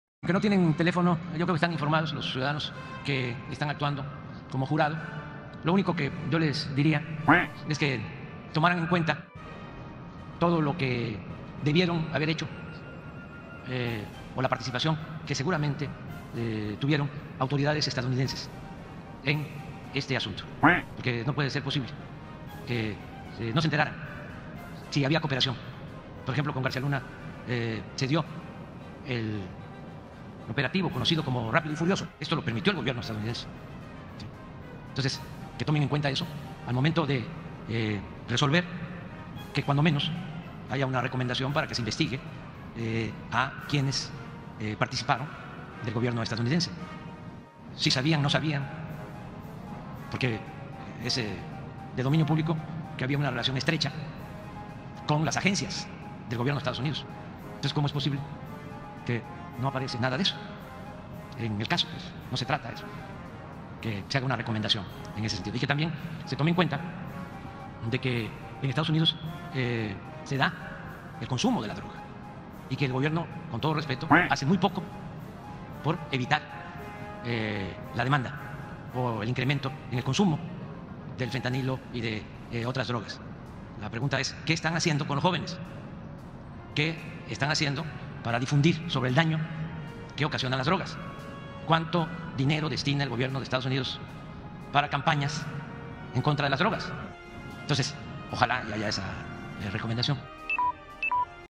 AMLO® le hace una recomendación al jurado del JucioGarcíaLuna 🦆🇺🇸
AMLO® le hace una recomendación al jurado del JucioGarcíaLuna, que tomen en cuenta la participación de las autoridades estadounidenses, y que se pregunten qué hace el gobierno de Estados Unidos, para evitar el consumo de droga 🦆